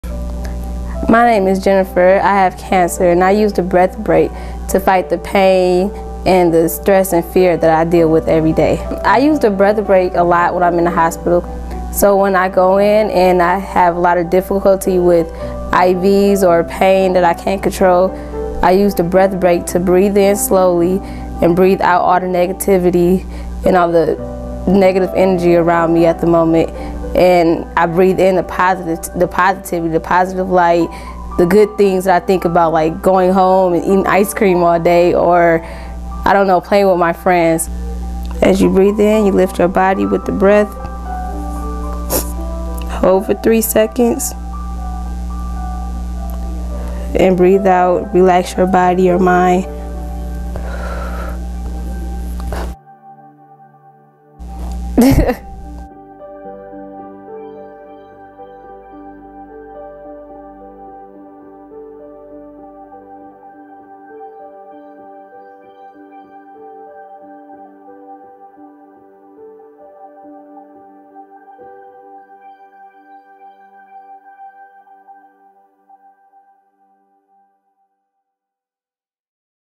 Jennifer.mov
One of our Kids Kicking Cancer students demonstrates how to do the "Breath Brake"™ to remove stress, pain and fear from our lives.